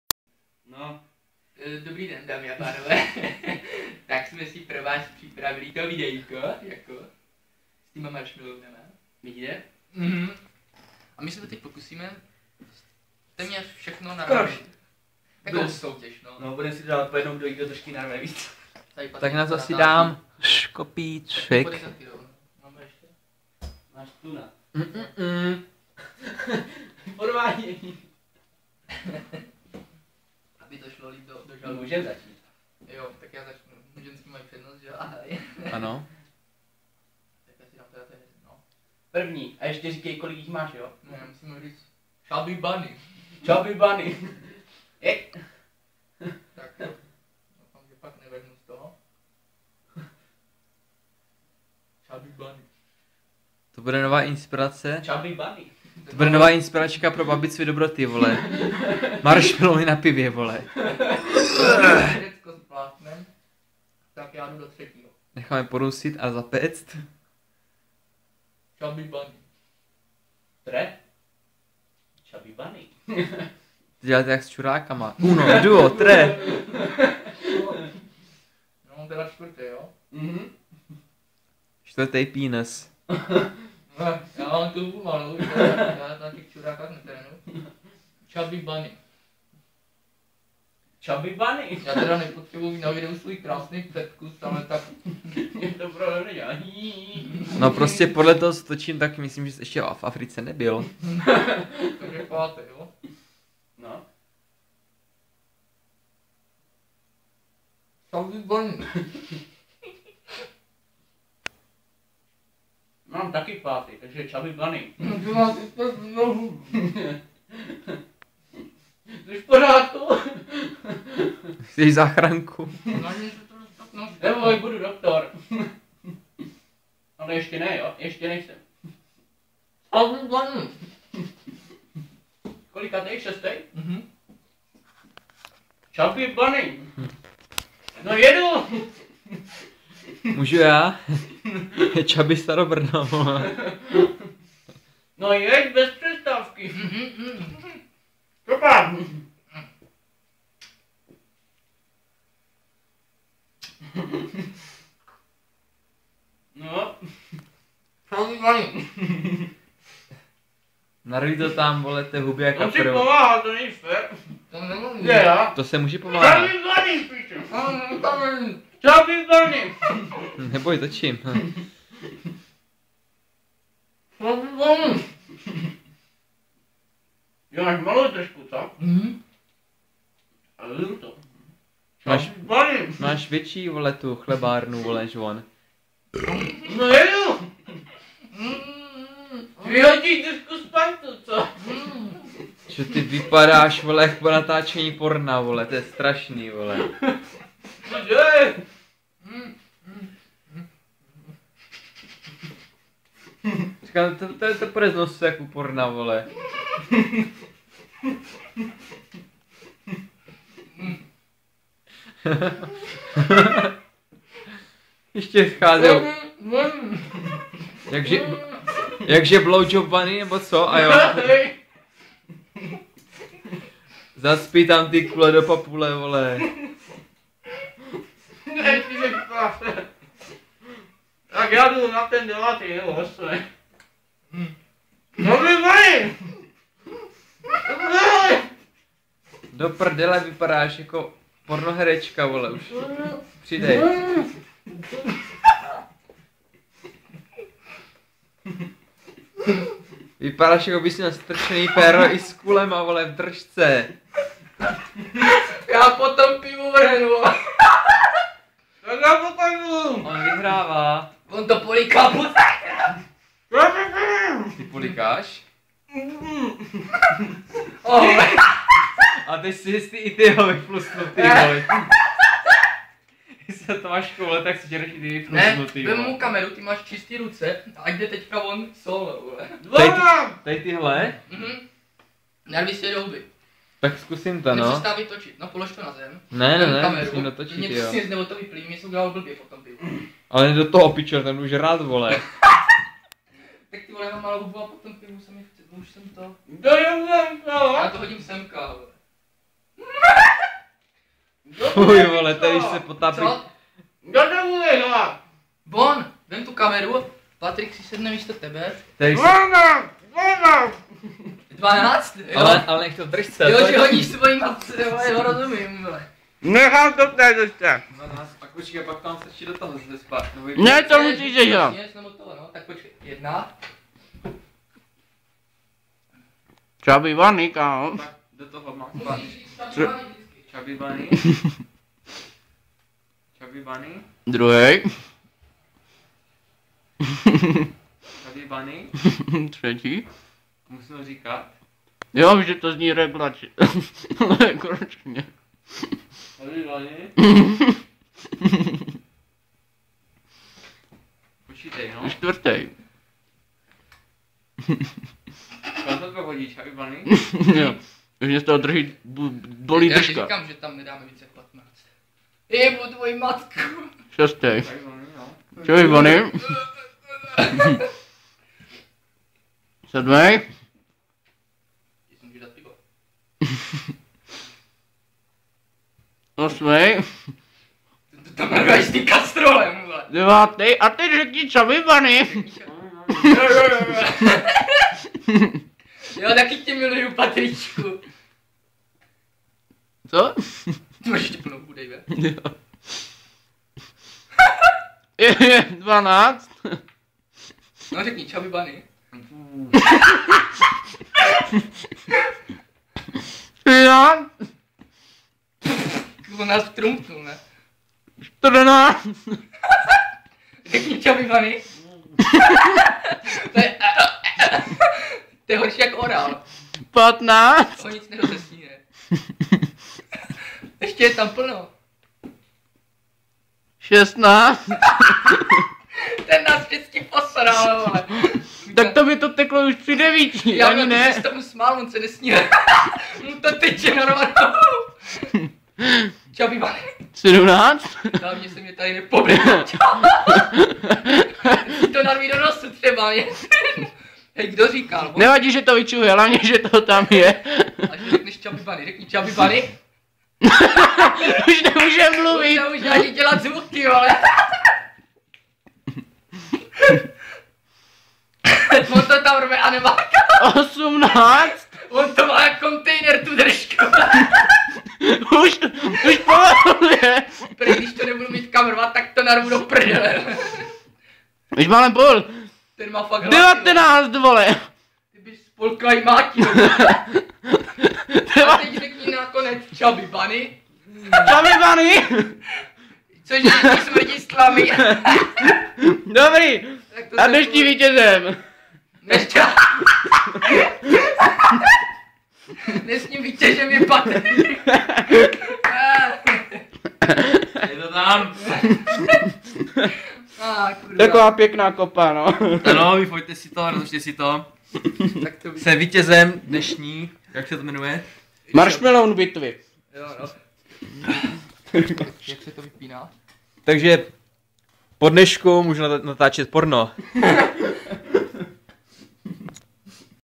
Maršmelouny na pivě ( Dawe & Sovis)
Souboj titanu kdo so držky nacpe vice maršmelounu
Nechceme nikoho kopirovat jen si užíváme srandy